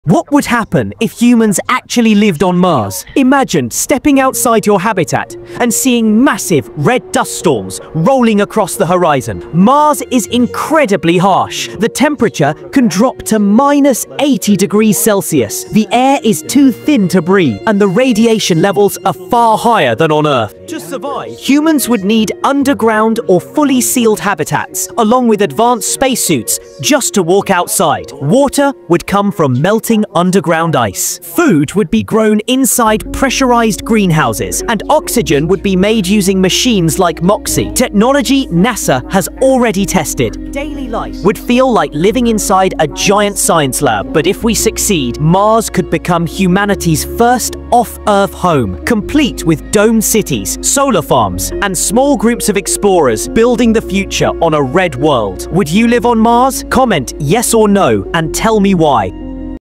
What If Humans Actually Lived on Mars?The Real Future Explained
What would life actually look like if humans lived on Mars? In this video, we explore the mind-blowing reality of surviving on the Red Planet — from deadly dust storms to futuristic habitats, underground farms, oxygen machines, radiation shelters, and the possibility of building humanity’s first off-Earth home.
This short explainer breaks it all down in a simple, cinematic, and easy-to-understand way.

I’m a student, and I don’t always have time to create everything manually, so some of the visuals in this video were generated using Sora 2. If you notice anything imperfect, please forgive me — but the voiceover is always 100% my own voice in every video.